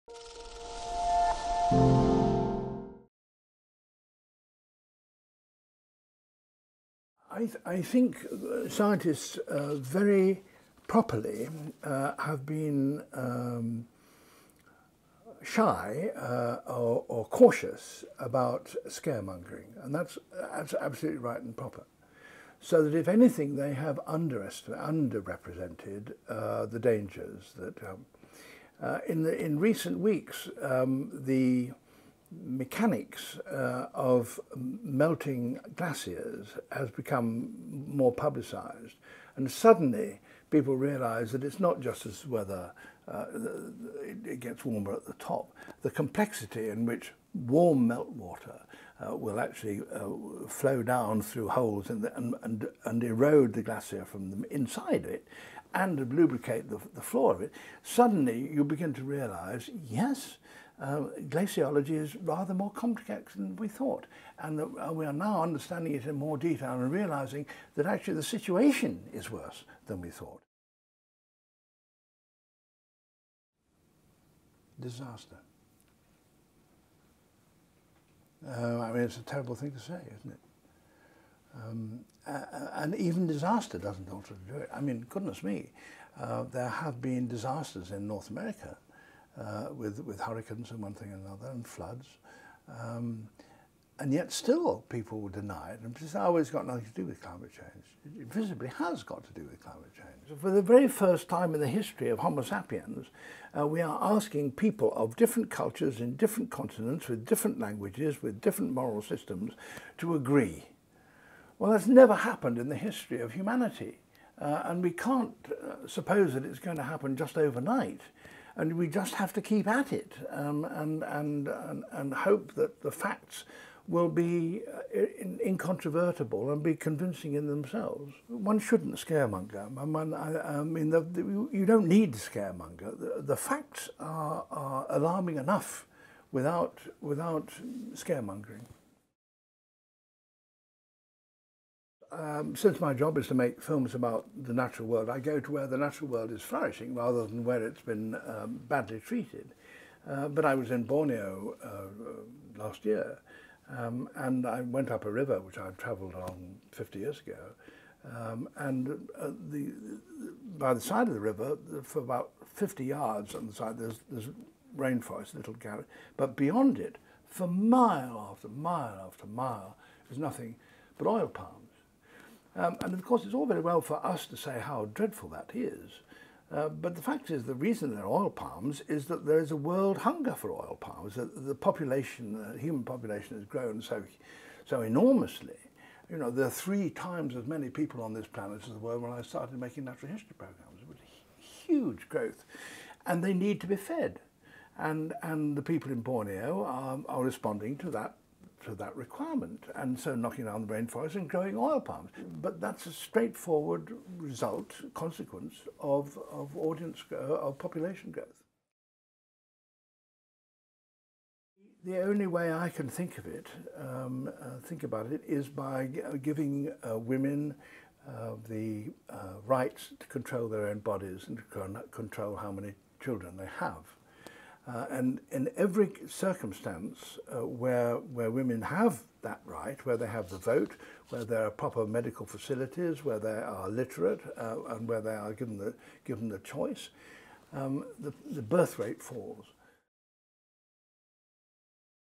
Sir David Attenborough: 'Climate change dangers worse than we thought'
The Guardian interviews Sir David Attenborough about the impact of climate change and population growth.

The broadcaster and naturalist says scientists and environmentalists have been cautious of overstating the dangers of global warming, but recent evidence of melting polar caps shows the situation is worse than had been thought. He also discusses population growth and disappearing habitats.